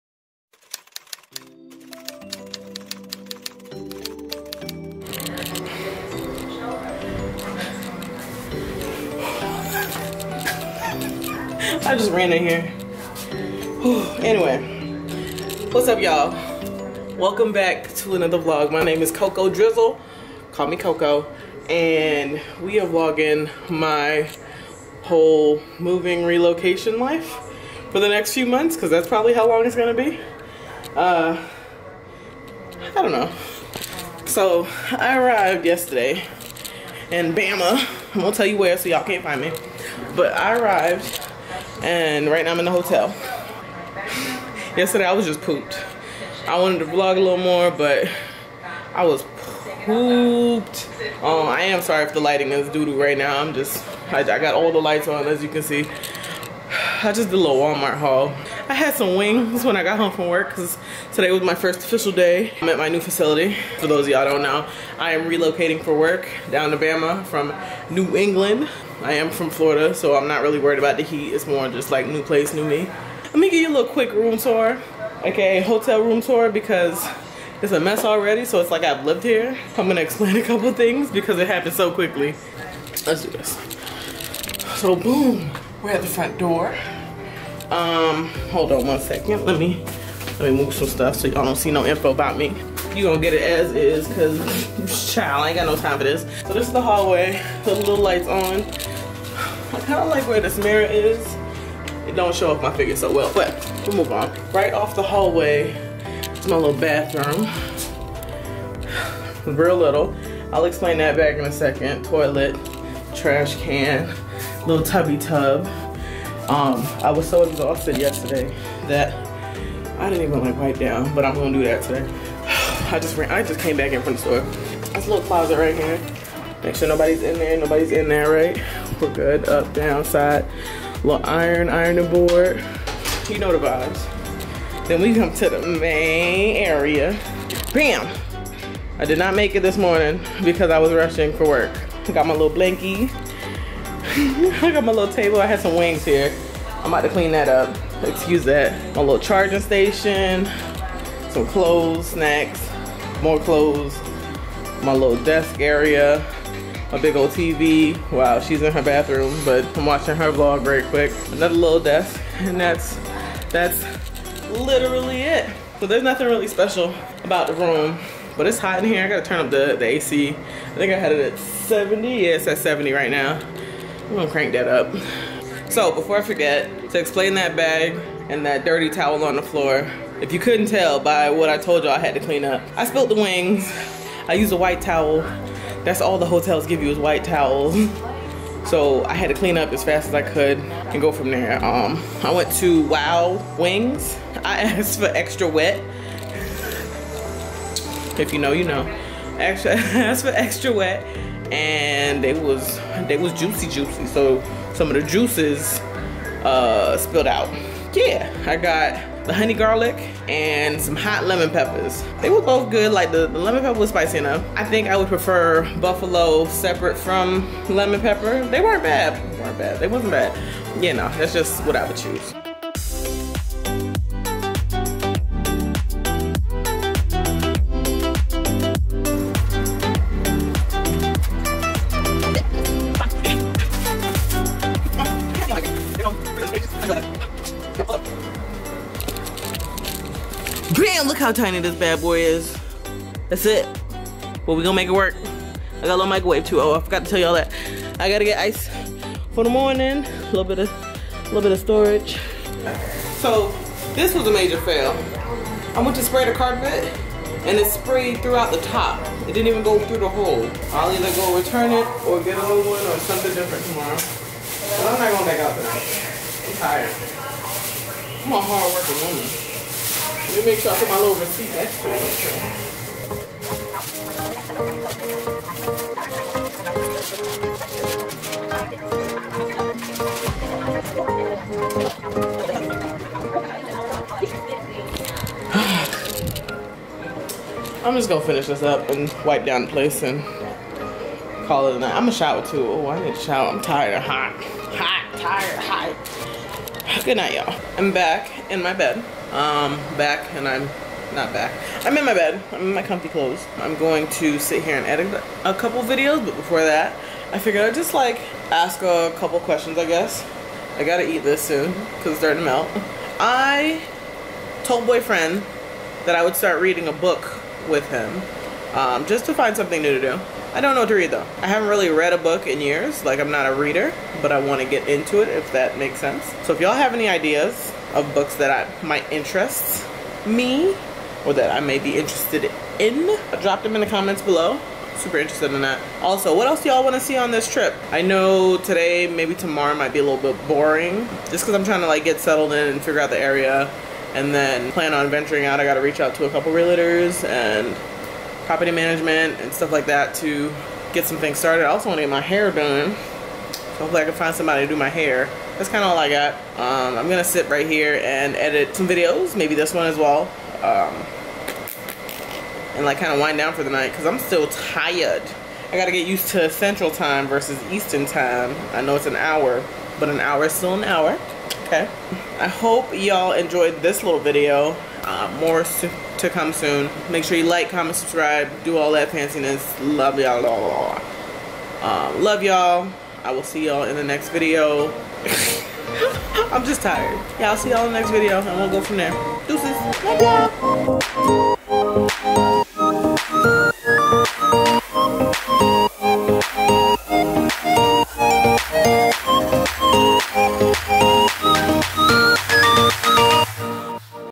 Moving Vlog | Hotel Tour It's just the beginning
Getting comfy for my extended stay in a hotel! Ignore the fact that I haven't caught my breath lmao.